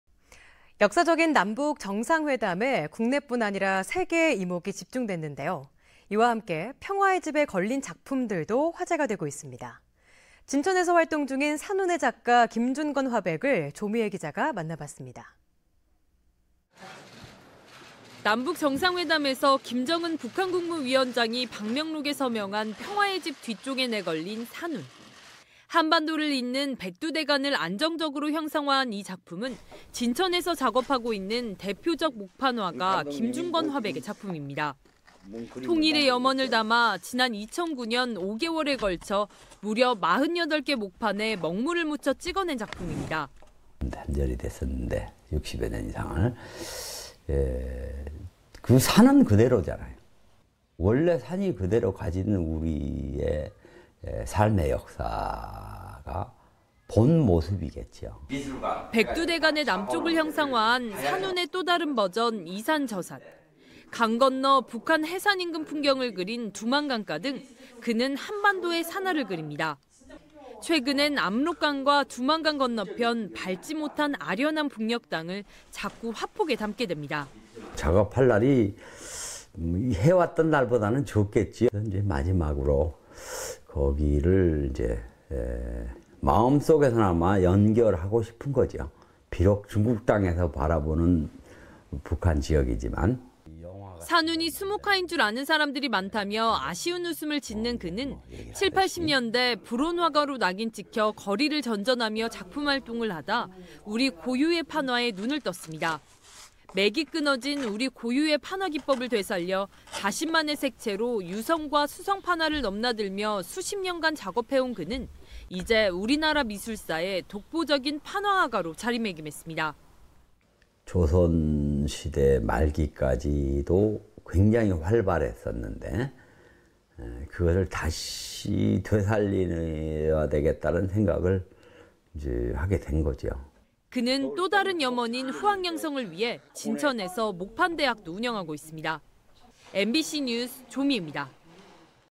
평화의 집에 걸린 판화 '산운' , 작가 김준권을 만나다ㅣ남북정상회담ㅣMBC충북 NEWS
[앵커]
역사적인 남북 정상 회담이
국내 뿐 아니라 세계의 이목이 집중됐는 데요,

 이와 함께 평화의 집에 걸린 작품들도
화제가 되고 있습니다.

 진천에서 활동 중인 '산운'의 작가
김준권 화백을 조미애 기자가 만나봤습니다.

[기자]
 남북정상회담에서 김정은 북한 국무위원장이
방명록에 서명한 평화의 집 뒤쪽에 내걸린
'산운.'

 한반도를 잇는 백두대간을 안정적으로
형상화한 이 작품은 진천에서 작업하고 있는
대표적 목판화가 김준권 화백의 작품입니다.

 통일의 염원을 담아 지난 2009년 5개월에 걸쳐 무려 48개 목판에 먹물을 묻혀 찍어낸
작품입니다.

[김준권/목판 화가]
"(남북 분단 이후) 단절이 됐었는데 60여 년 이상을. 그 산은 그대로잖아요. 원래 산이 그대로 가지는 우리의 삶의 역사가 본모습이겠죠."

 백두대간의 남쪽을 형상화한 '산운'의
또다른 버전 '이산 저산', 강 건너 북한 혜산 인근 풍경을 그린 '두만강가' 등 그는
한반도의 산하를 그립니다.

 최근엔 압록강과 두만강 건너편 밟지 못한
아련한 북녘 땅을 자꾸 화폭에 담게 됩니다.

[김준권/목판 화가]
"작업할 날이 해왔던 날보다는 적겠죠. 마지막으로 거기를 마음속에서나마 연결하고 싶은 거죠. 비록 중국 땅에서 바라보는 북한 지역이지만"

 '산운'이 수묵화인 줄 아는 사람들이 많다며
아쉬운 웃음을 짓는 그는, 70,80년대
'불온 화가'로 낙인 찍혀 거리를 전전하며
작품 활동을 하다 우리 고유의 판화에 눈을 떴습니다.

 맥이 끊어진 우리 고유의 판화 기법을 되살려
자신만의 색채로 유성과 수성 판화를 넘나들며 수십 년간 작업해온 그는 이제 우리나라 미술사에 독보적인 판화 화가로 자리매김했습니다.

  [김준권/목판 화가]
"조선 시대 말기까지도 굉장히 활발했었는데 그거를 다시 되살려야 되겠다는 생각을 하게 된 거죠."

 그는 또다른 염원인 후학 양성을 위해
진천에서 목판 대학도 운영하고 있습니다.
 MBC뉴스 조미애입니다.